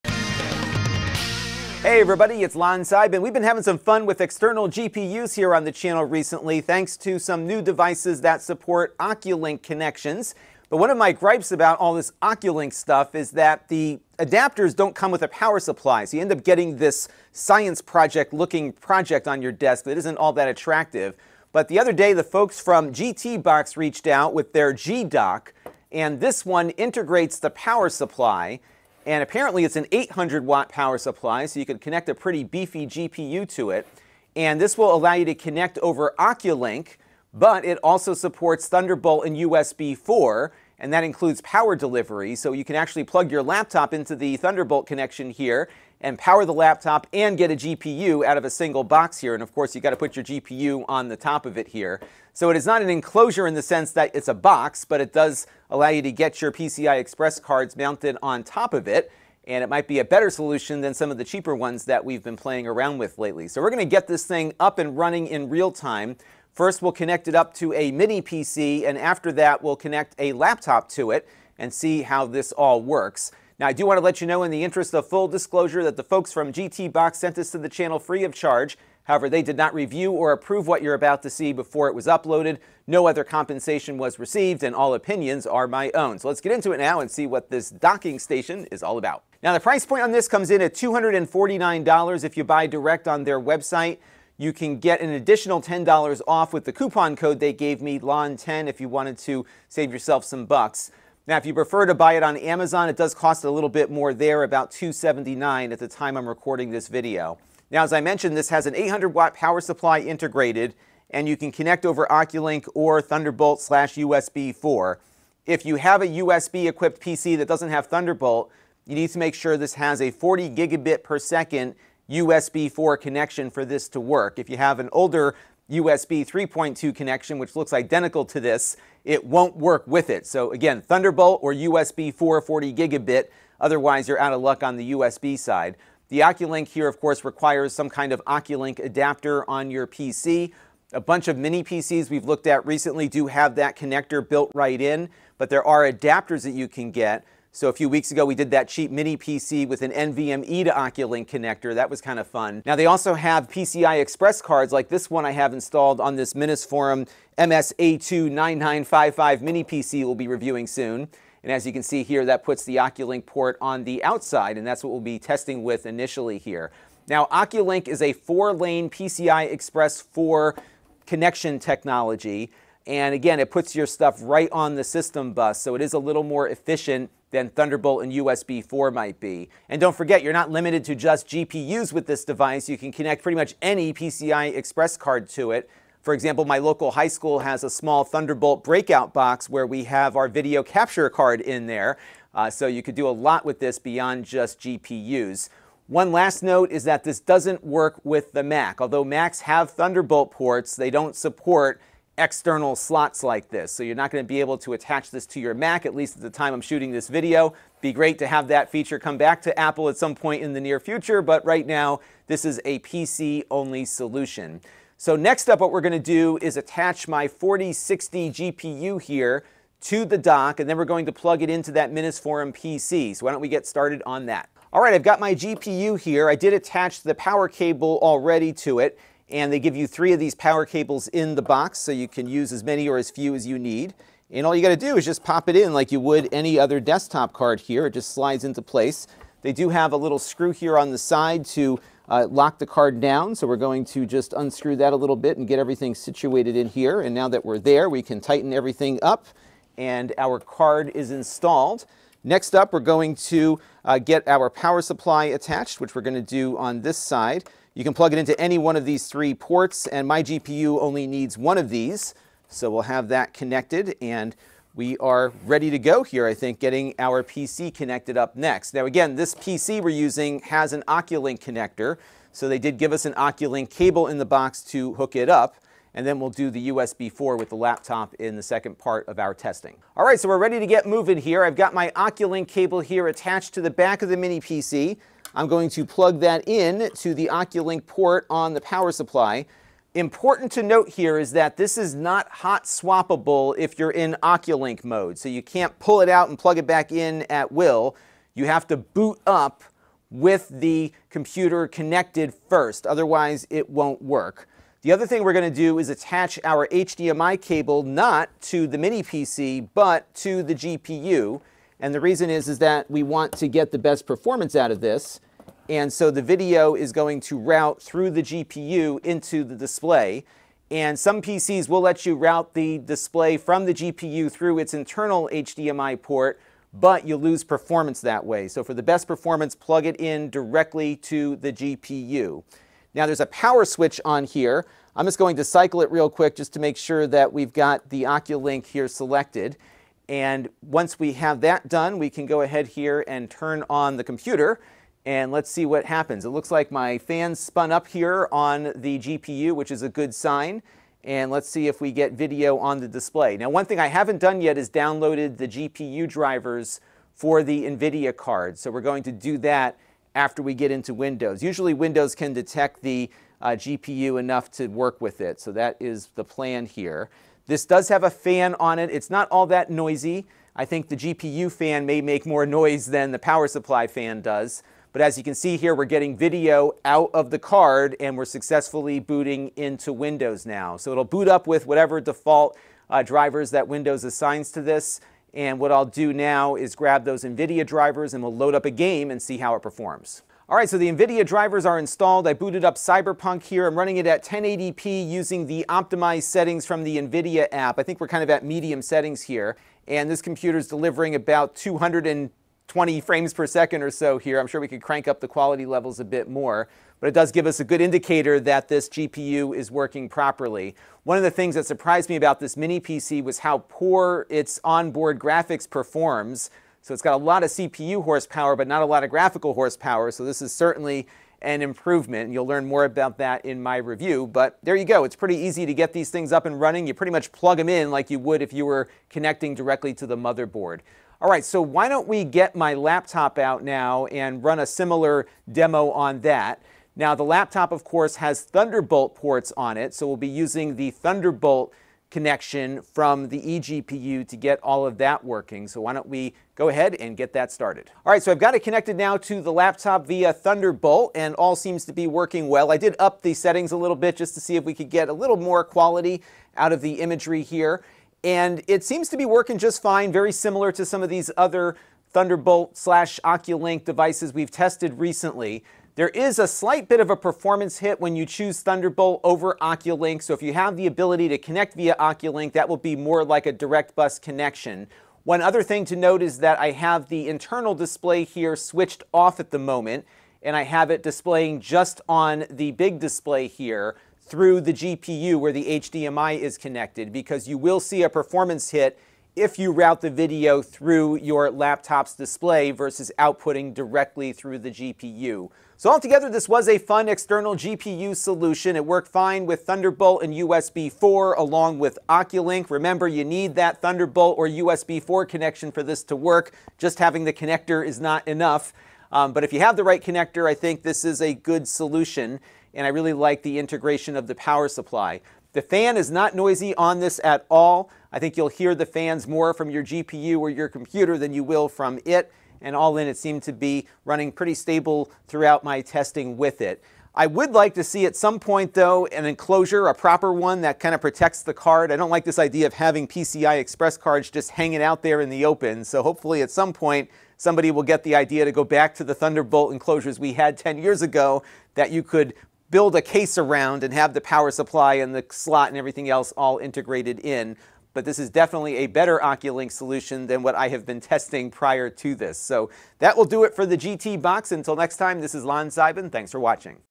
GTBox G-Dock Review - eGPU Dock for Oculink/Thunderbolt/USB 4 With Integrated Power Supply!
Buy it direct and save $10 with code LON10 also find it on Amazon - The GTBox G-Dock is an eGPU enclosure with an integrated 800 watt power supply.

VIDEO INDEX:
00:00 - Intro
01:29 - Price
01:47 - PC Requirements
02:46 - Oculink Technology
03:15 - No Mac Compatibility
03:44 - GPU installation
04:39 - Attaching PC to the Dock
06:29 - Oculink game demo
07:27 - Thunderbolt / USB 4 test
08:15 - Conclusion

Want to chat with other fans of the channel? our Discord